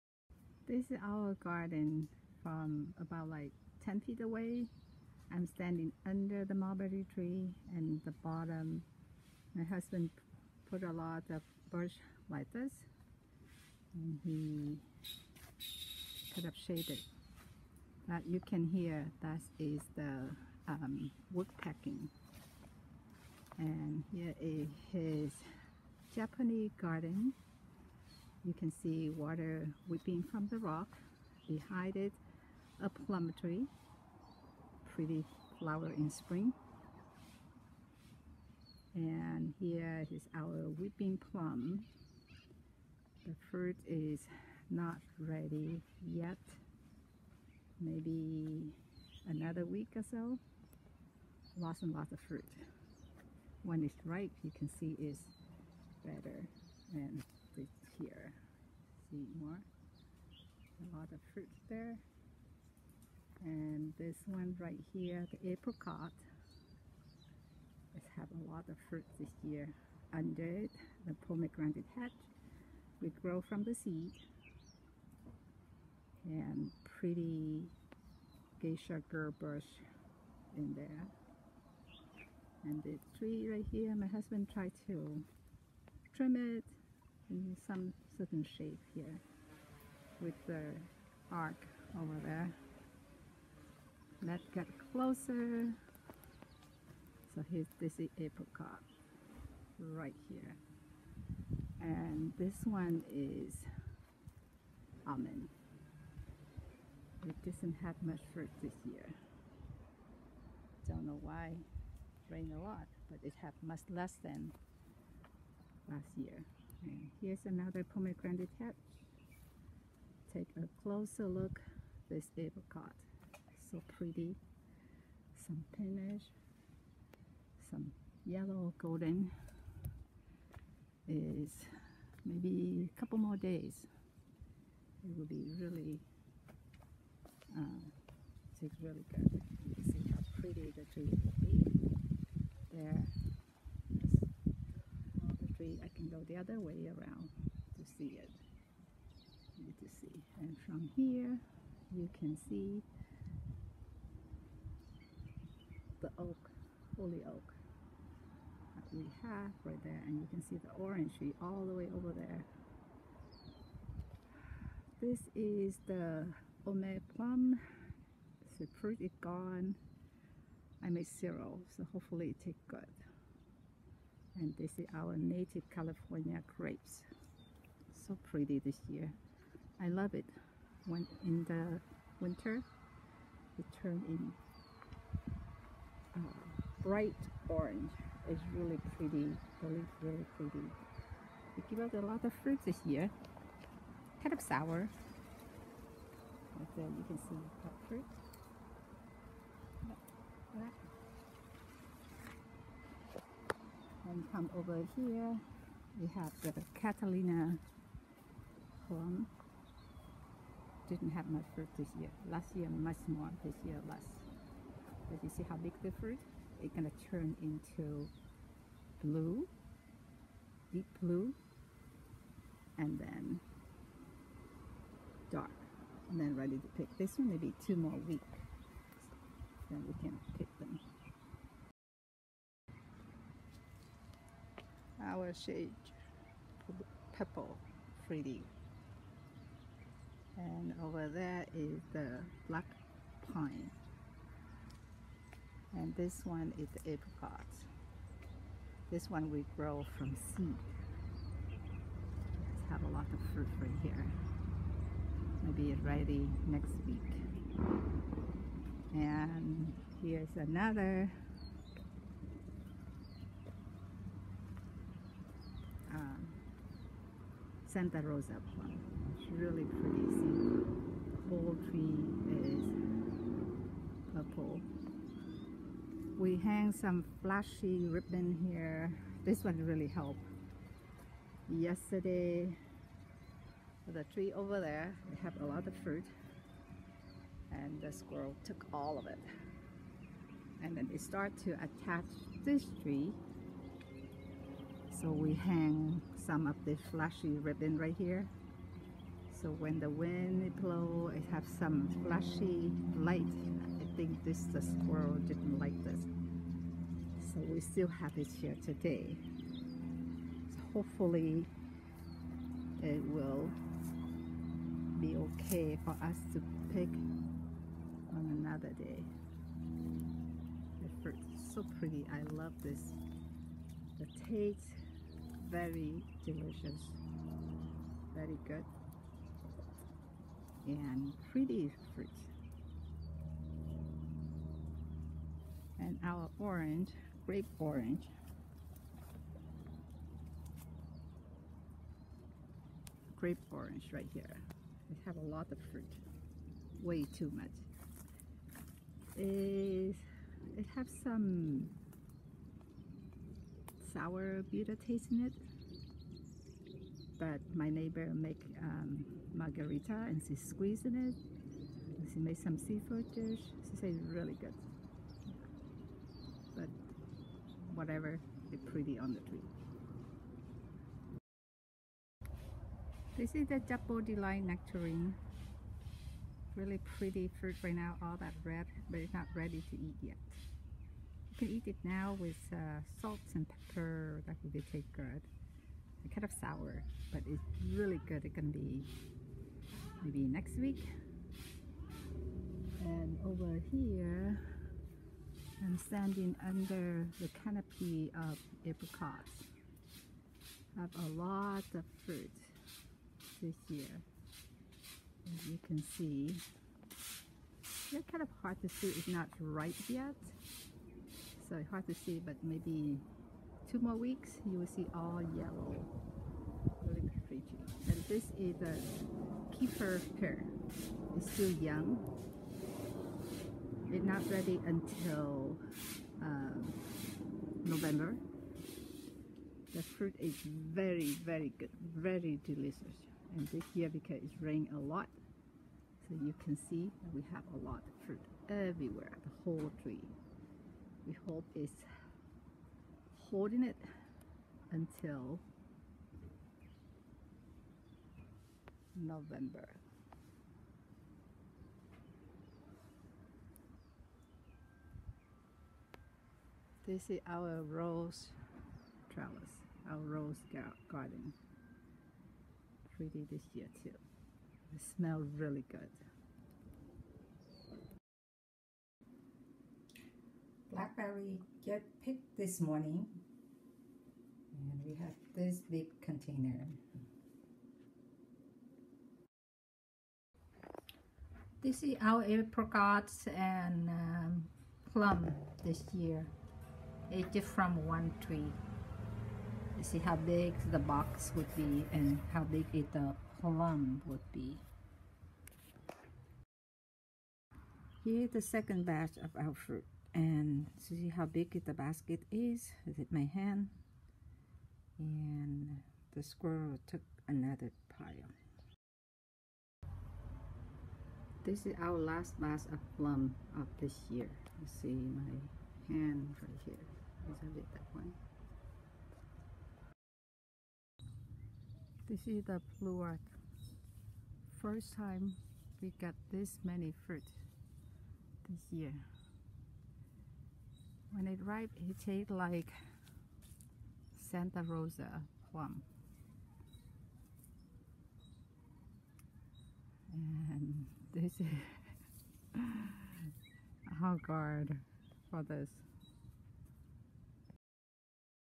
A Walk through a Bountiful Fruit Tree Garden in Los Angeles
Recorded from Jun 21 to July 21